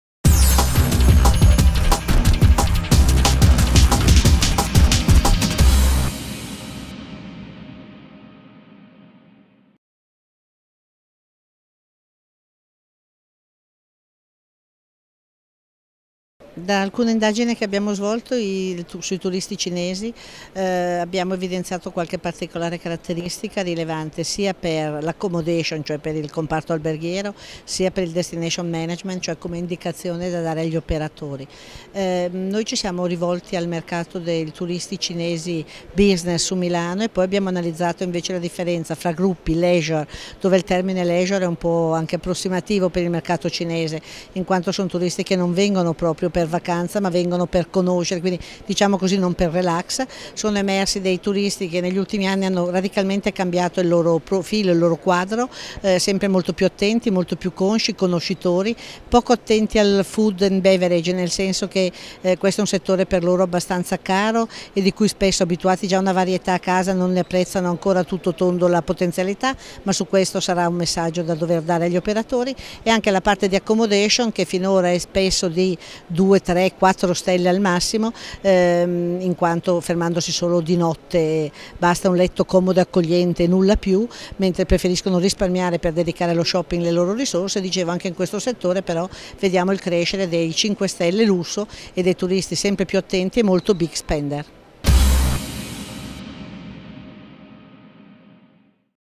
BIT2013 - Il Turismo Outbound Cinese
Intervista a Magda Antonioli Corigliano - Master Economia del Turismo -- Università Bocconi

L'ospitalità italiana al servizio del turista dagli occhi a mandorla